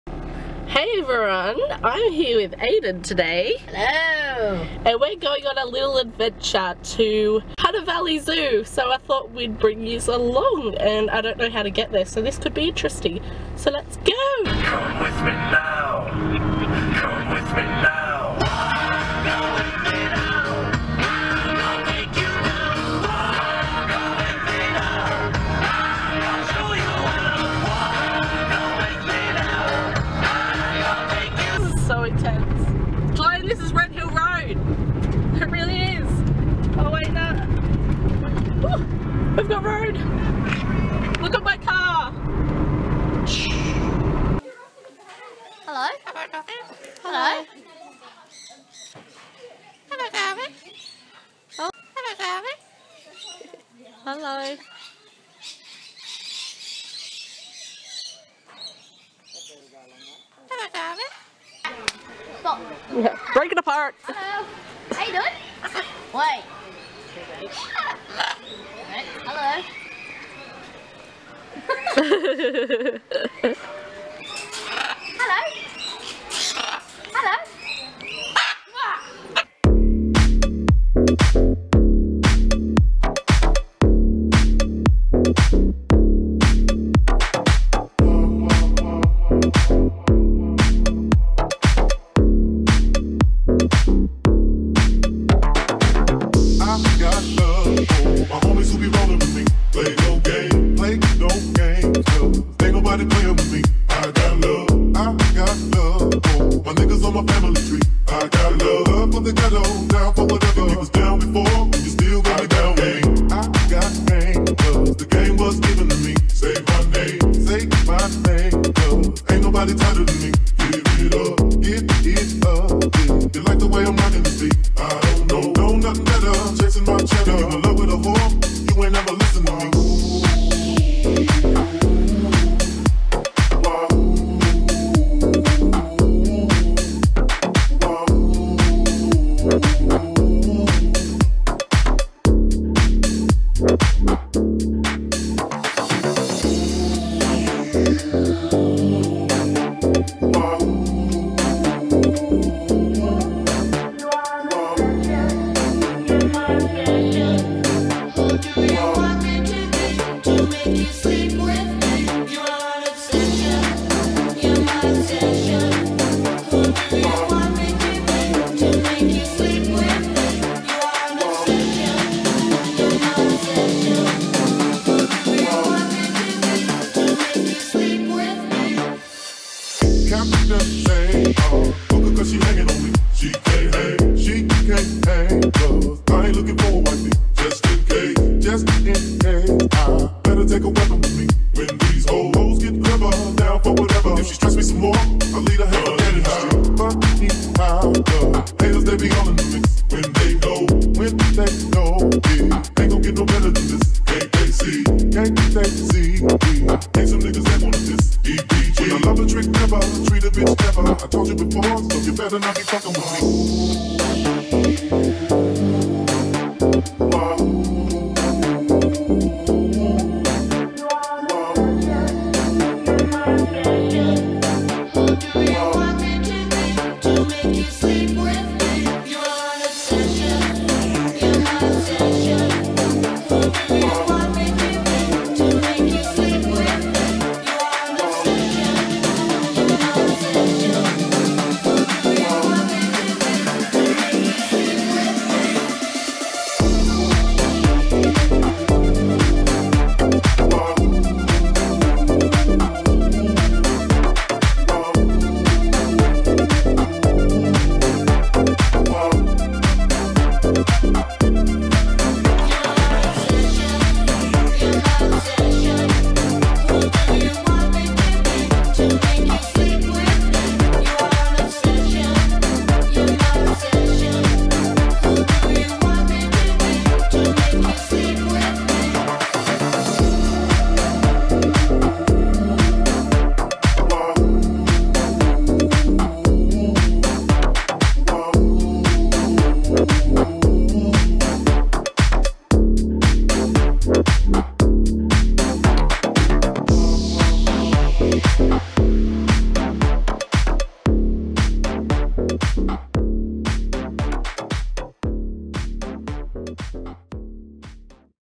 Hunter Valley Zoo 2014
Aydyn and I decided to spontaneously go to Hunter Valley Zoo and thought we would vlog along the way!

Instagram + Twitter : @kjcarbpd